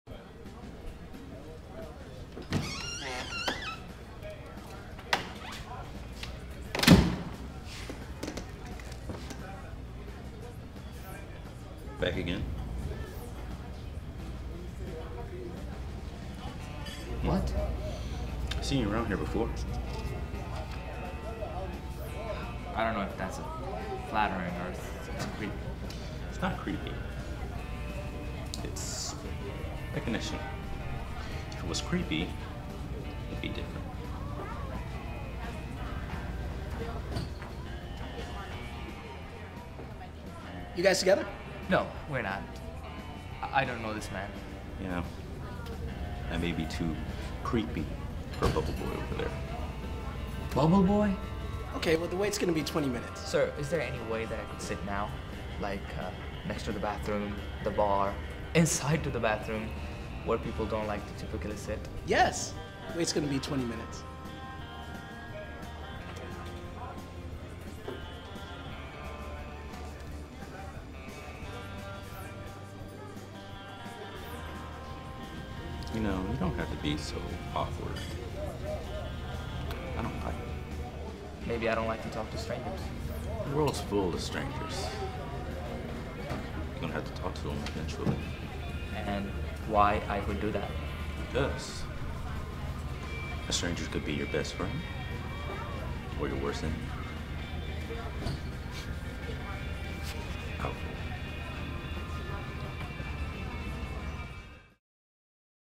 Strangers | Surreal Drama Scene "Stalker" (2019)
Inspired By my own creation "Alfred From Technical"

Logline: Alfred comes into contact with a man he fears may be stalking him.

Writen, Directed, and Edited By Dimitri Luttrell
Director of Photgraphy Jordan Creel

Alfred: Bishal Sidartha
Stranger: Sir'Quentin Ross
Waiter: Jordan Johnson

Full Sail Scene Study for APN